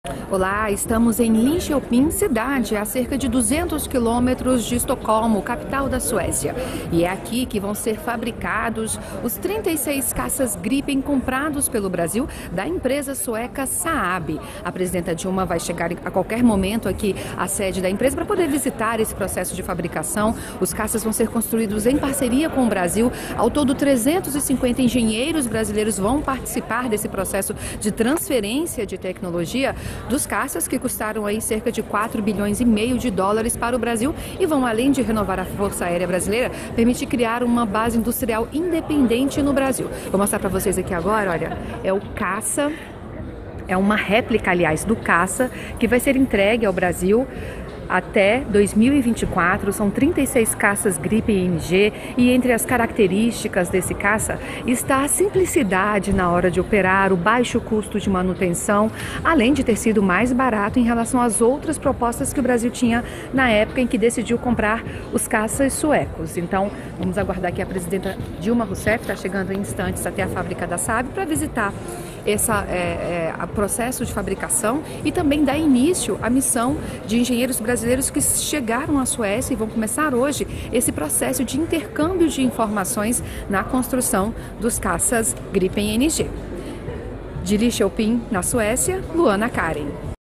Dilma Rousseff visita empresa fabricante dos caças ‪‎Gripen
ESPECIAL WEB - 19.10.15: Em seu último compromisso oficial à Suécia, Dilma Rousseff segue para Linköping. Na cidade que fica a 200 km de Estocolmo, a presidenta visitará a empresa fabricante dos caças ‪#‎Gripen‬, comprados pelo Brasil.